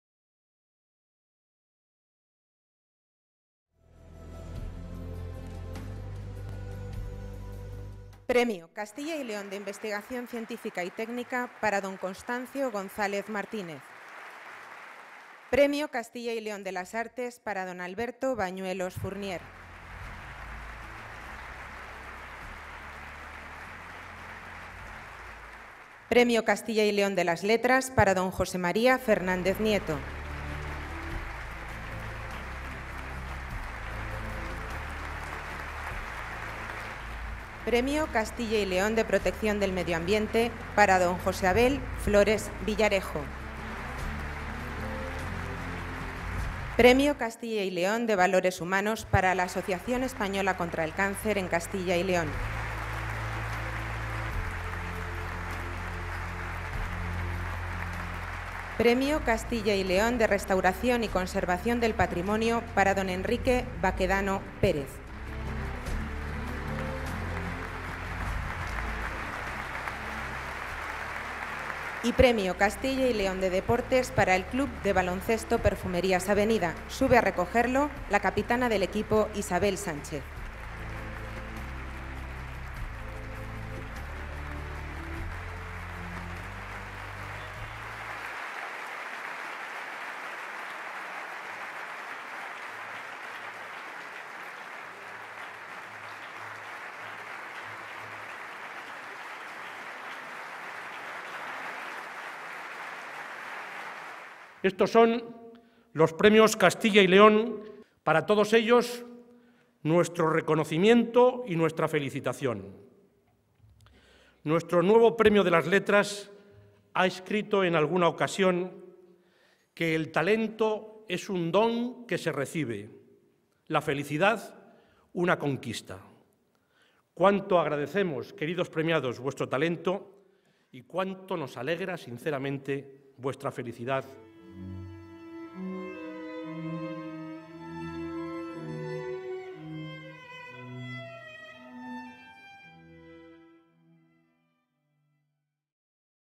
Entrega de los Premios de Castilla y León 2011
El presidente de la Junta de Castilla y León, Juan Vicente Herrera ha hecho entrega de los Premios Castilla y León, que en su edición 2011 han correspondido a Domingo Emilio Rodríguez Almeida, en la categoría de Ciencias Sociales y Humanidades; a José María Fernández Nieto, en la categoría de Letras; al Club Baloncesto Perfumerías Avenida, en la categoría de Deporte; a la Asociación Española contra el Cáncer en Castilla y León, en el apartado de Valores Humanos; a Alberto Bañuelos Fournier, en el de las Artes; a Constancio González Martínez, en el de Investigación Científica y Técnica; a Enrique Baquedano Pérez, en el apartado de Restauración y Conservación del Patrimonio, y a José Abel Flores Villarejo, como Premio a la Protección del Medio Ambiente.